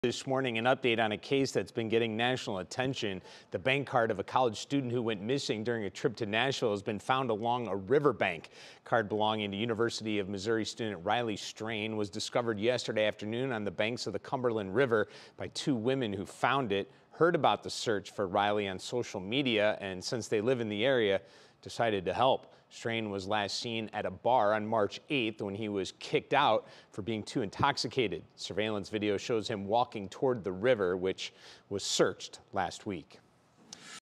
Search for Riley Strain continues in Nashville: His bank card found near Cumberland River
The Metro Nashville Police Department reported that a bank card belonging to Riley Strain was found near the Cumberland River on Sunday afternoon.

Police said his card was found on the embankment between Gay Street and the Cumberland River. According to WSMV4 Nashville, Strain’s family confirmed that the card does belong to Strain.

The University of Missouri student went missing in Nashville on March 8 and was last seen stumbling around downtown Nashville on security camera.